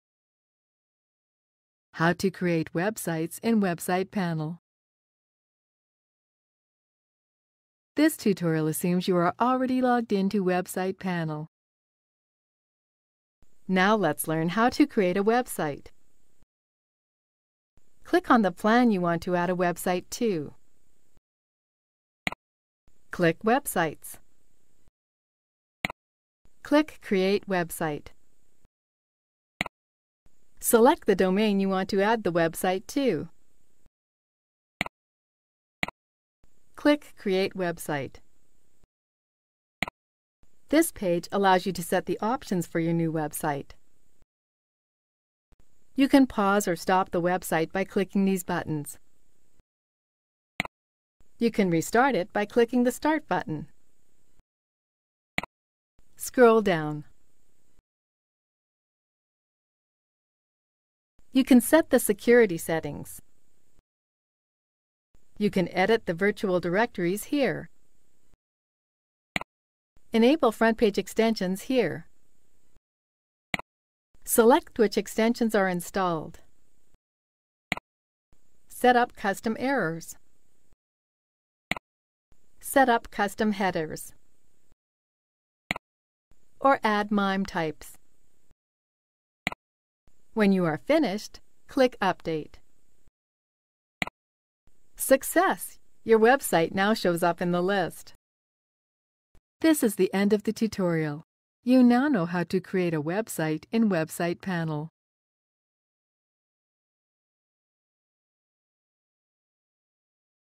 How to create Web Sites in WebsitePanel
• Log into WebsitePanel.
• Click the plan on which you wish to add website.
• Click Web Sites tab from left side of window. Click Create Web Site button.
• Select the domain to which you want to add website from drop down menu. Click Create Web Site button.
• You will get options for your new site. You can pause or stop website by clicking on the pause and stop icons at top. It can be restarted by clicking play icon.
• Fill options and click on Update button. Also, select other tabs one by one and make required additions. Update the changes.
• Your website will show up in the list.